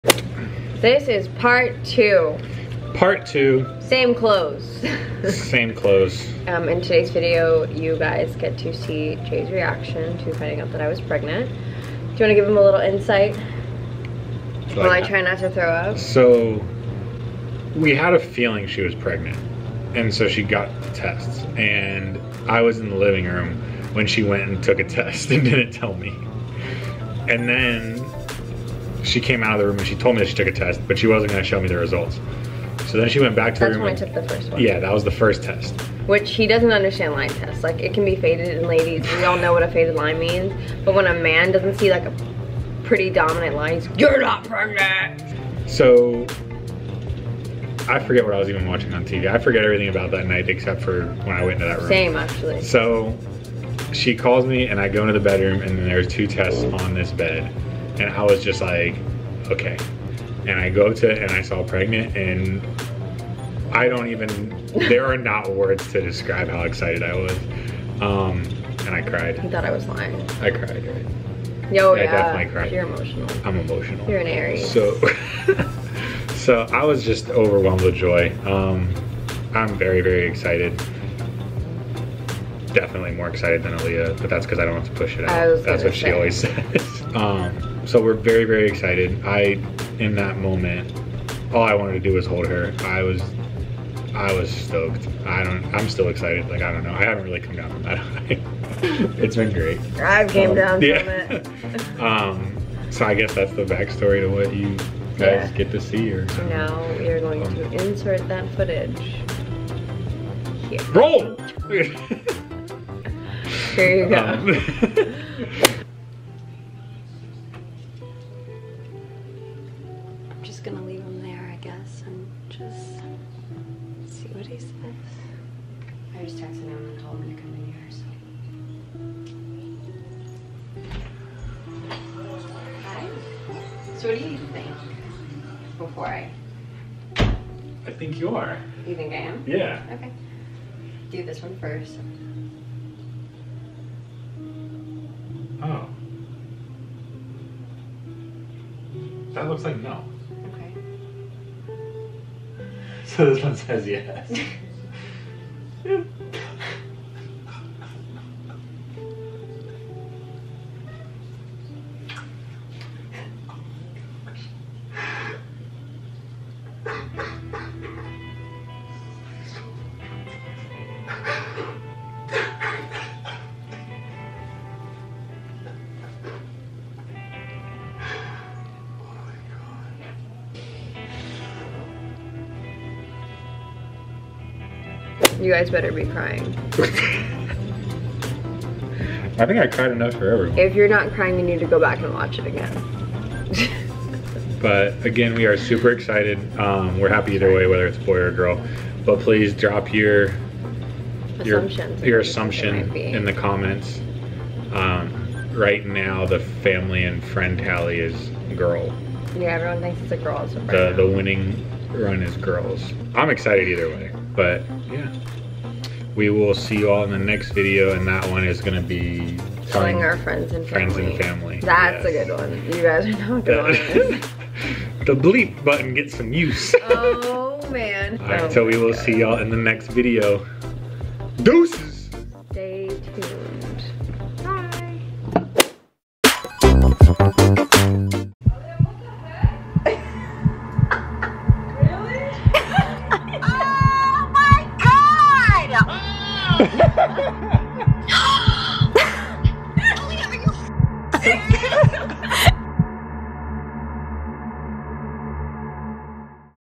TELLING MY BOYFRIEND I'M PREGNANT *he bawls*!!!!
GRAB YOUR TISSUES, THIS ONE IS EMOTIONAL!

Baby Fischer 💛 ‘s Baby Registry